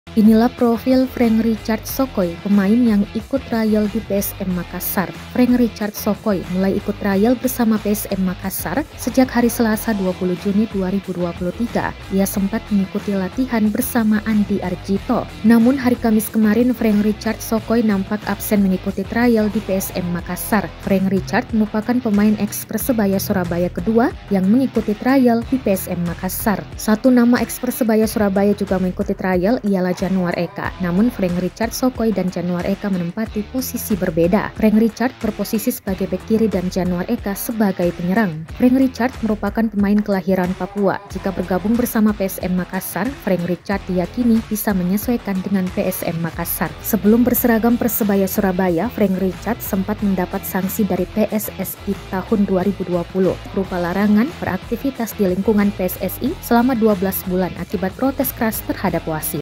SHORT | Frank Richards Sokoy, Eks Persebaya dan Barito Trial di PSM Makassar
TRIBUN-TIMUR.COM - Profil Frank Richards Sokoy pemain ikut trial di PSM Makassar.

Frank Richards Sokoy mulai ikut trial bersama PSM Makassar sejak hari Selasa (20/6/2023).

Ia sempat mengikuti latihan bersama Andy Arjito.

Namun hari Kamis kemarin, Frank Richards Sokoy, nampak absen mengikuti trial di PSM Makassar.

Frank Richard Sokoy merupakan pemain eks Persebaya Surabaya kedua mengikuti trial di PSM Makassar.

Satu nama eks Persebaya Surabaya juga mengikuti trial ialah Januar Eka.

Namun Frank Richard Sokoy dan Januar eka menempati posisi berbeda.

Frank Richard Sokoy berposisi sebagai bek kiri dan Januar Eka sebagai penyerang.

Dilansir transfermark, harga pasaran Frank Richard Sokoy ditaksir Rp1,74 M.

Siapa Frank Richards Sokoy?

Frank Richards Sokoy merupakan pemain kelahiran Papua.

Jika bergabung bersama PSM Makassar, Frank Richards Sokoy, diyakini bisa menyesuaikan dengan PSM Makassar.

Frank Richards punya rekan sesama asal Papua lebih dulu membela PSM Makassar.

Keduanya Yakob Sayuri dan Yance Sayuri.

Narator : Ririn
VP : Asrul